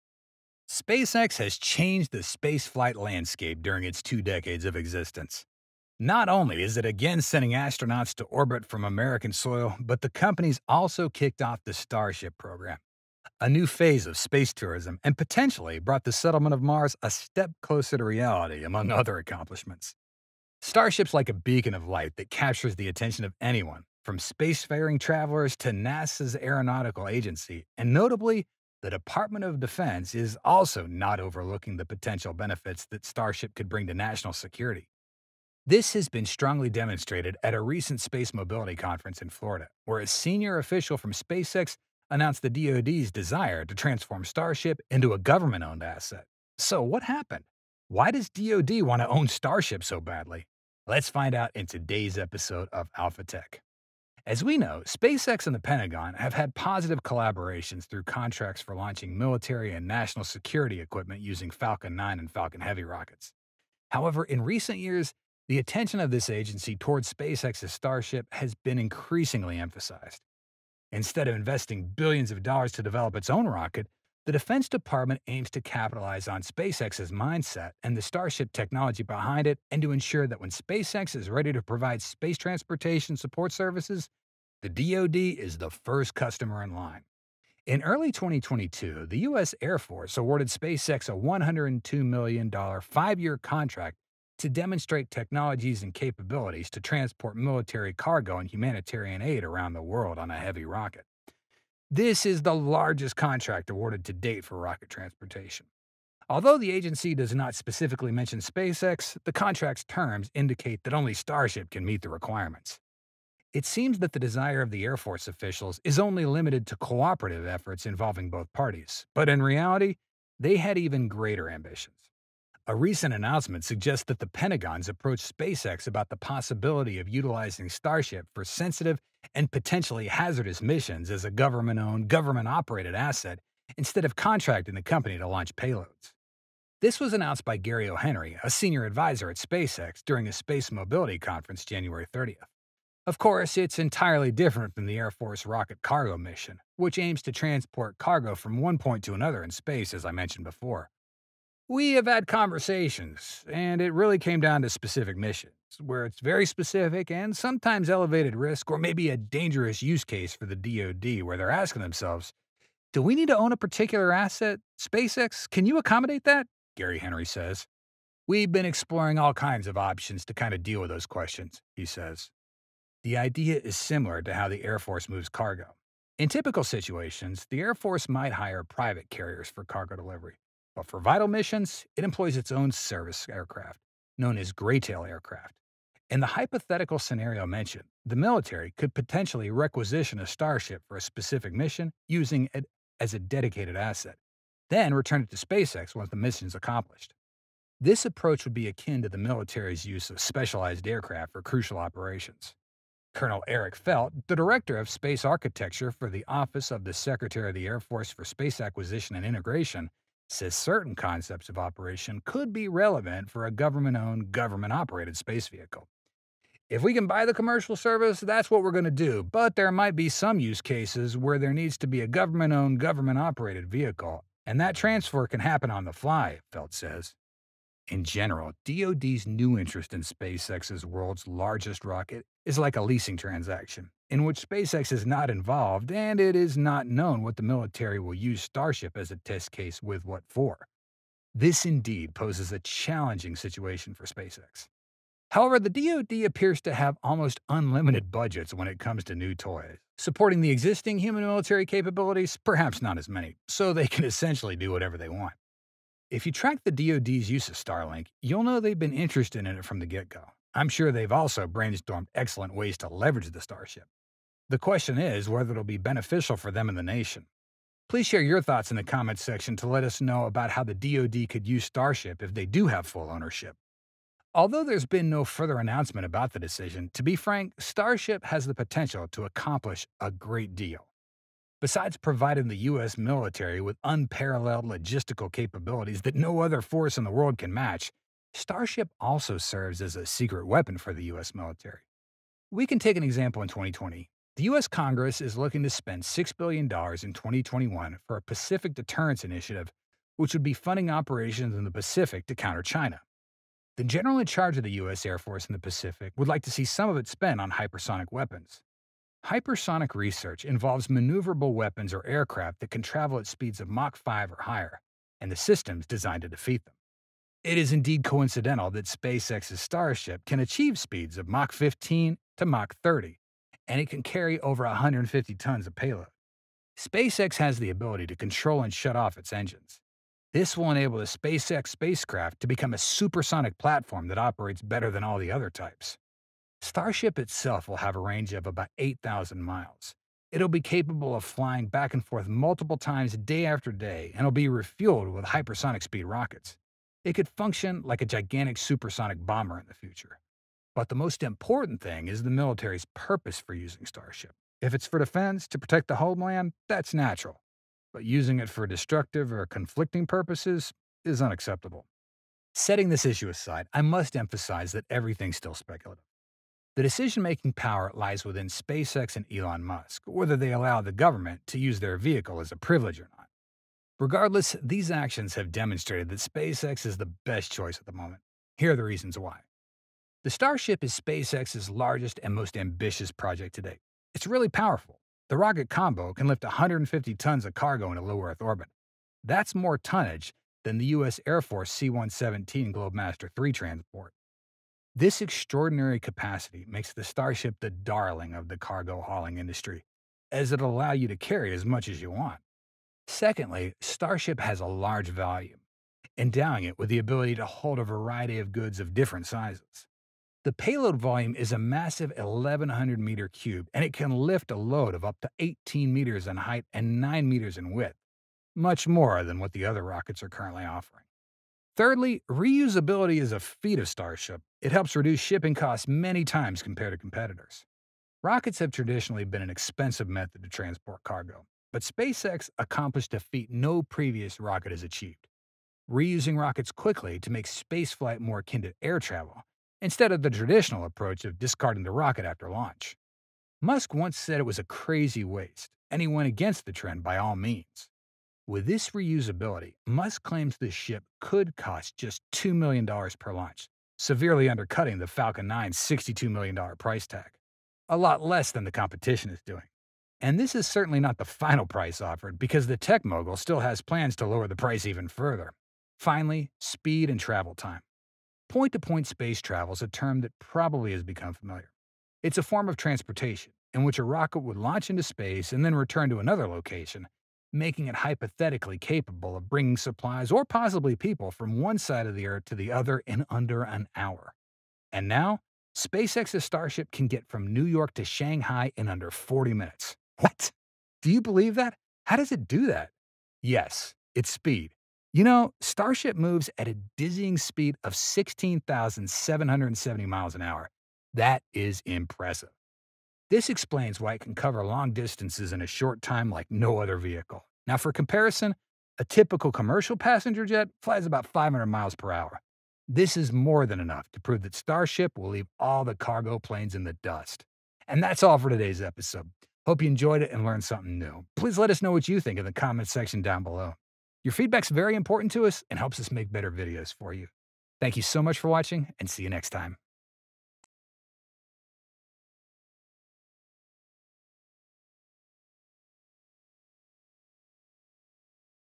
This INSANE! DoD Pentagon wants to OWN Elon Musk's Starship for the dangerous mission...
Sources of image & video:
Not only is it once again sending astronauts to orbit from American soil but the company has also kicked off the Starship program- a new phase of space tourism and potentially brought the settlement of Mars a step closer to reality, among other accomplishments.
Starship is like a beacon of light that captures the attention of anyone, from spacefaring travelers to NASA aeronautical agency, and notably, the Department of Defense is also not overlooking the potential benefits that Starship could bring to national security.
This has been strongly demonstrated at a recent Space Mobility Conference in Florida, where a senior official from SpaceX announced the DoD's desire to transform Starship into a government-owned asset.
So, what happened?
Why does DoD want to own Starship so much?